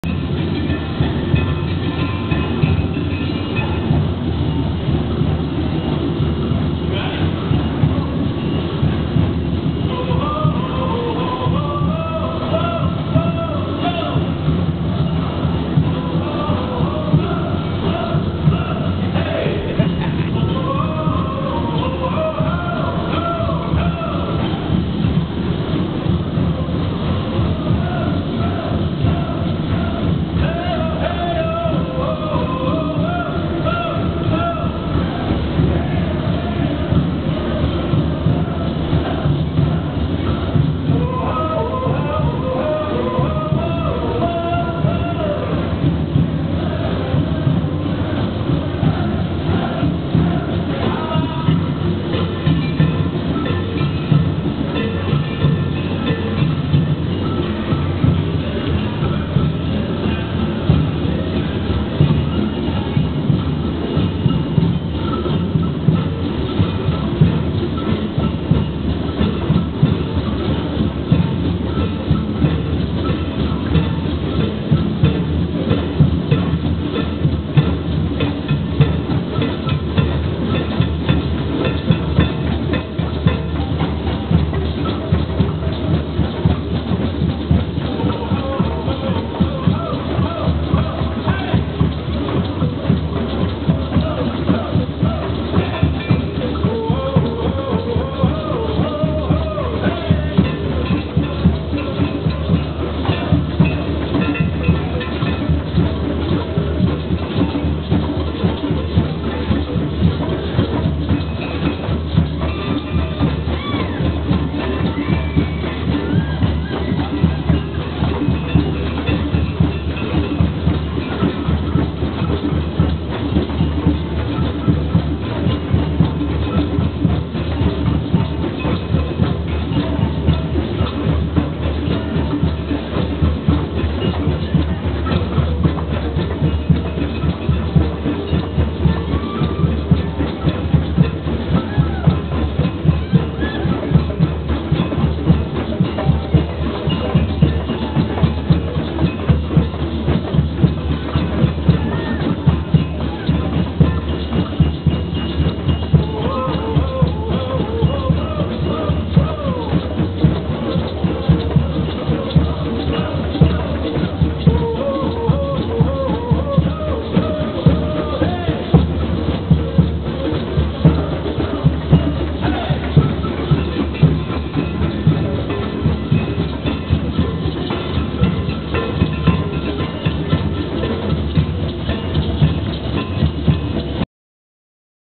aarp drum circle orlando
Remember those sunnyafternoons on Sproul Plaza? I recaptured some of that feeling at the end of each day at the AARP Orlando@50+ event.